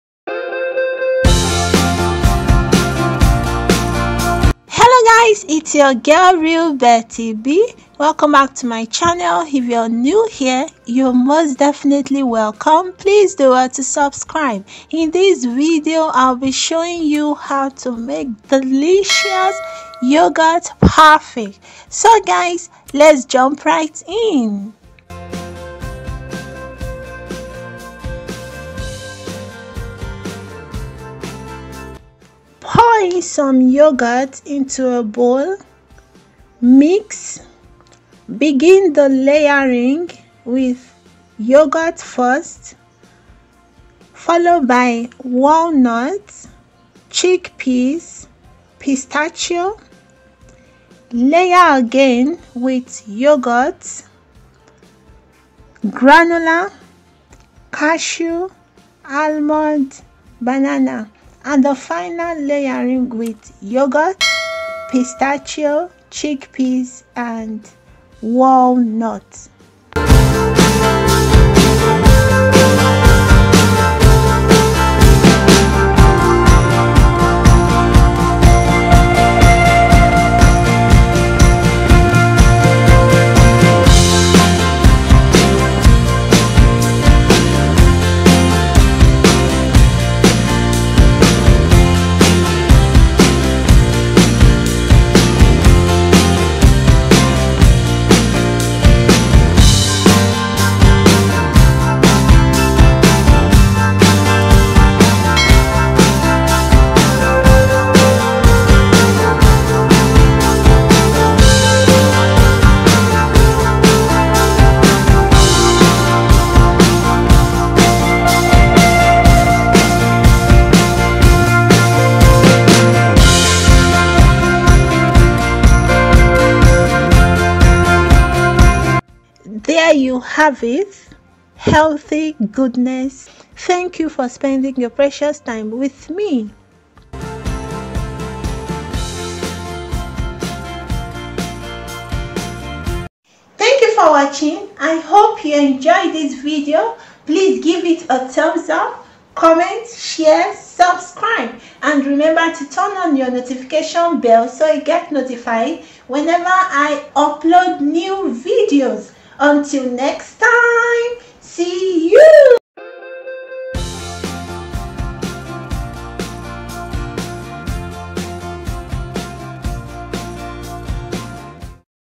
8 Recipes To Make Healthy Yoghurt Parfait | Natural Yoghurt
8 Recipes To Make Healthy Yoghurt Parfait | Natural Yoghurt

This is a recipe you'll definitely want to stick to.

RECIPE BENEFITS!
Yoghurt: Enhances weight regulation
Banana: Supports digestion and serves as energy booster
Granola: Increases energy & lowers blood pressure
Pistachio nuts: Highly rich in protein & promotes weight loss
Cashew nuts: Lowers blood sugar level
Almonds nuts: Rich in vitamins & minerals
Walnuts: Improves bone health
Chickpeas: An excellent source of fiber & also controls appetite

INGREDIENTS!
Natural yoghurt
Banana
Granola
Pistachio
Cashew
Almonds
Walnuts
Chickpeas